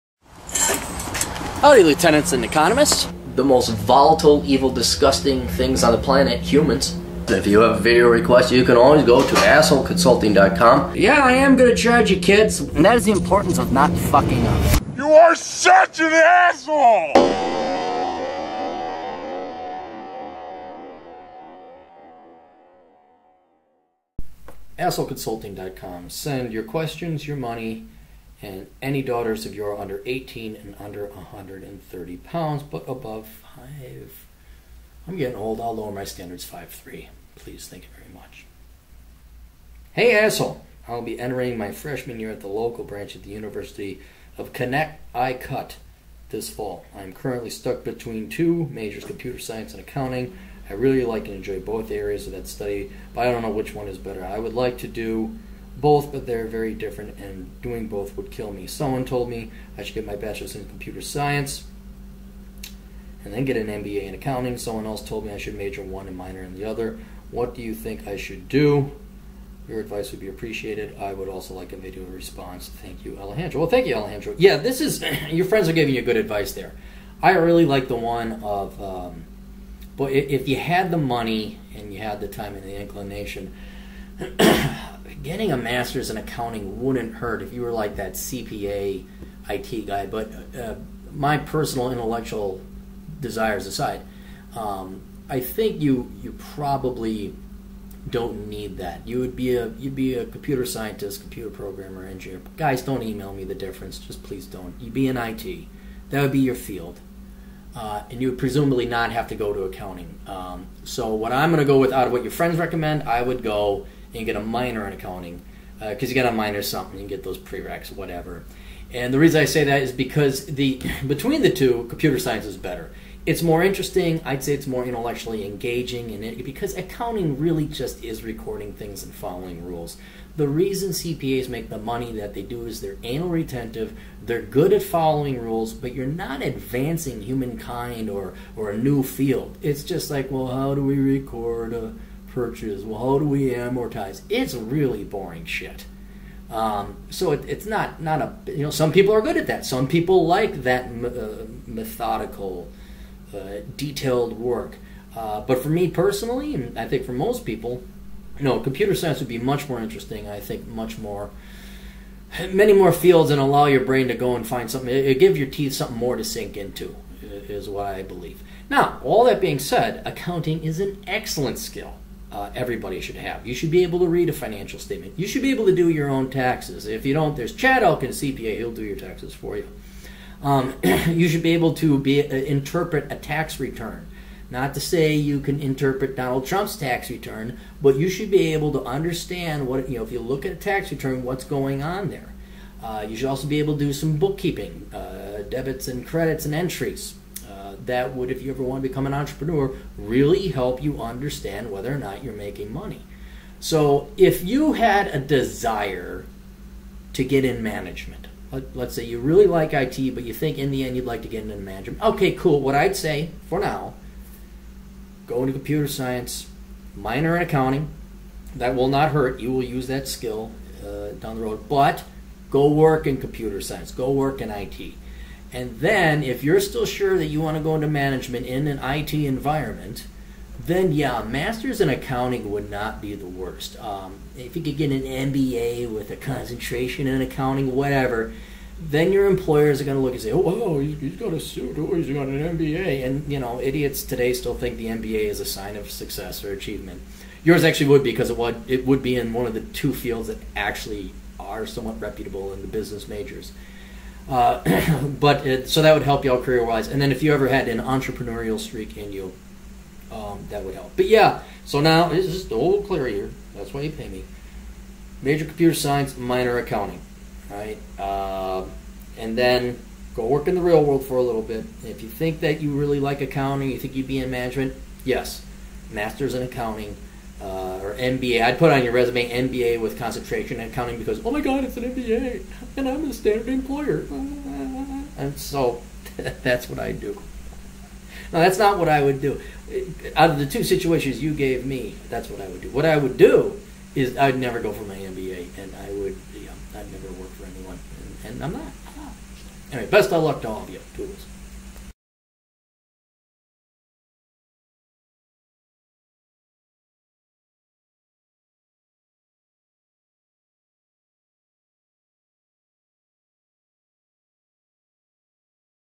Request-STEM or Accounting?
A college student has an interest in both Computer Science and Accounting, but doesn't know which is better.  Computer Science is, but that doesn't mean accounting wouldn't play a vital and helpful role in your future career.

The truth your father and brother were too big of pussies to give: